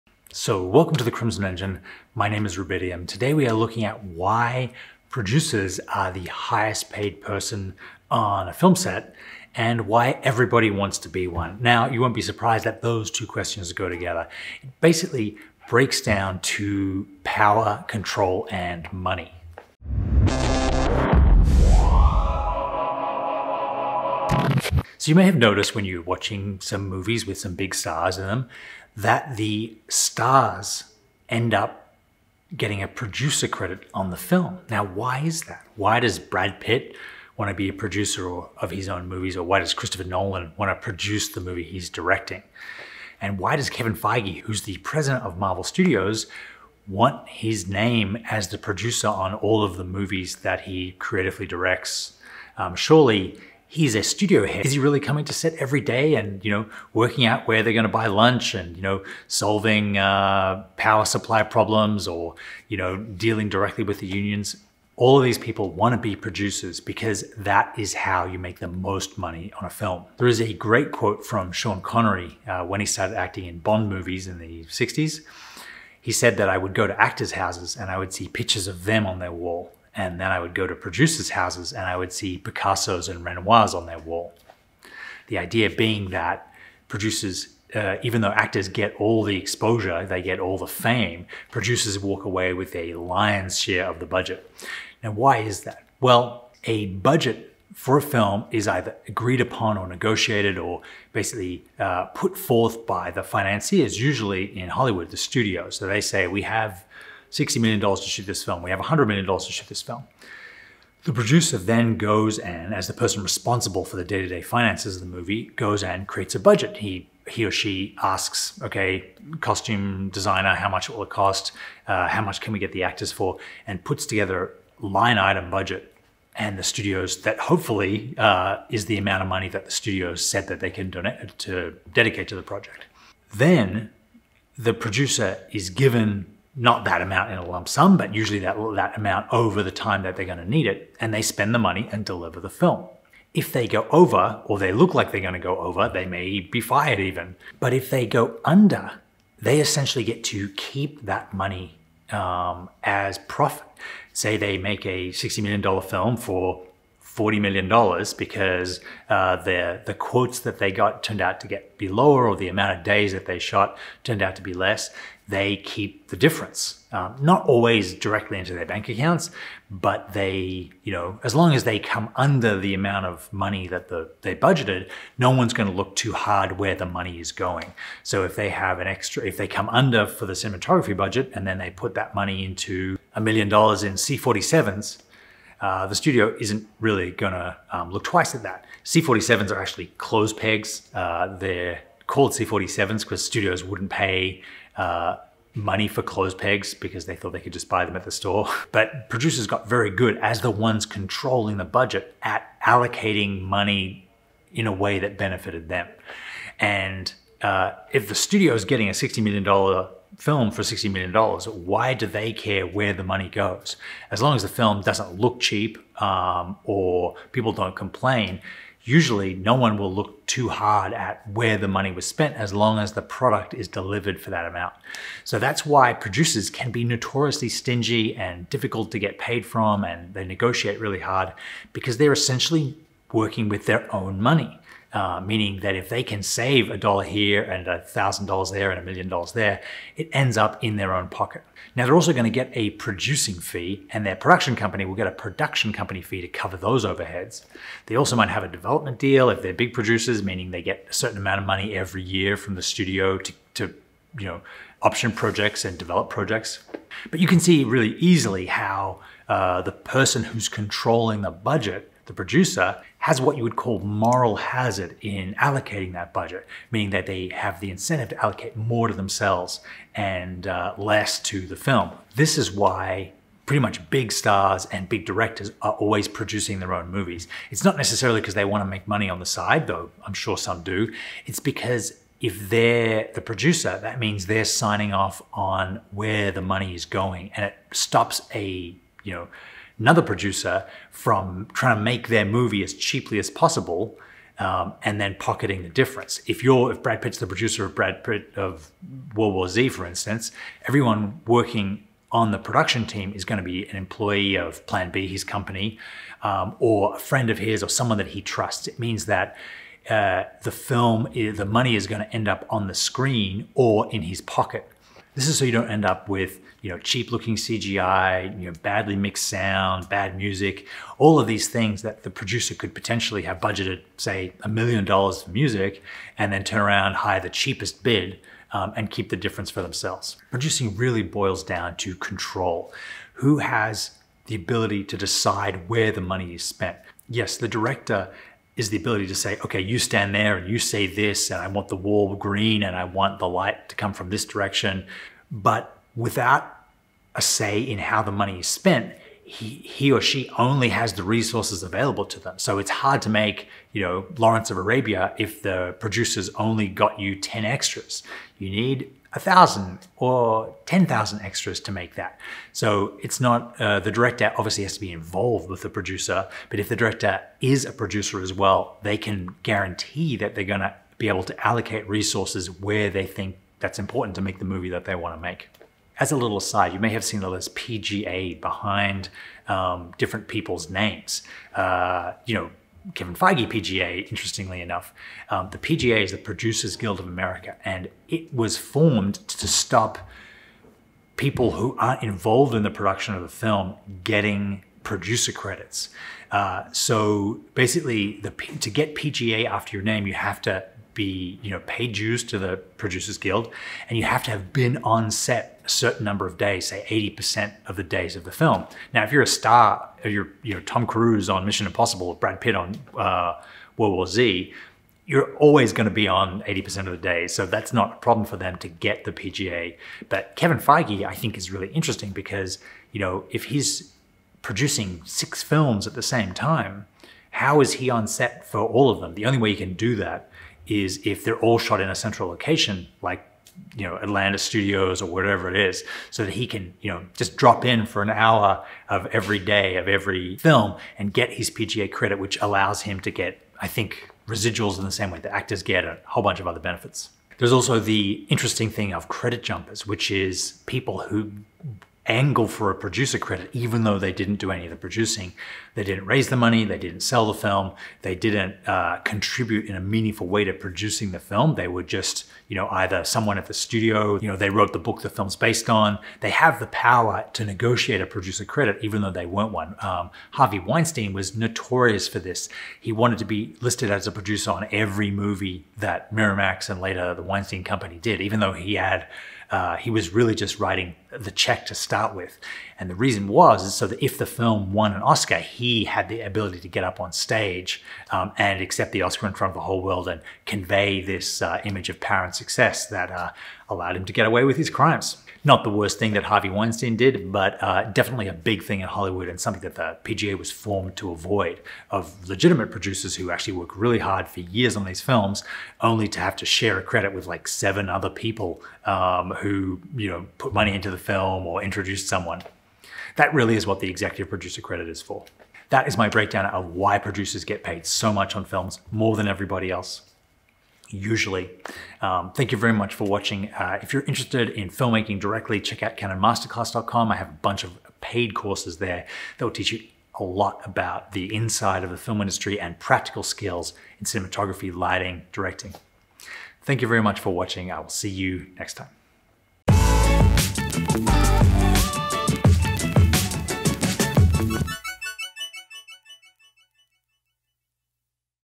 How Producers OUT EARN everyone in the Film Business
Why are Producers usually the highest paid person on a film set? What makes their contribution so valuable? Are power, control and money such significant influences?
🅾 𝙁𝙄𝙇𝙈𝙈𝘼𝙆𝙄𝙉𝙂 𝙏𝙊𝙊𝙇𝙎
🅾 𝙍𝙐𝘽𝙄𝘿𝙄𝙐𝙈'𝙎 𝙎𝙀𝙏𝙐𝙋
Main Camera
A-Cam Lens
Memory Cards
Key Light
🅾 𝘾𝙍𝙄𝙈𝙎𝙊𝙉 𝙀𝙉𝙂𝙄𝙉𝙀
Canon Masterclass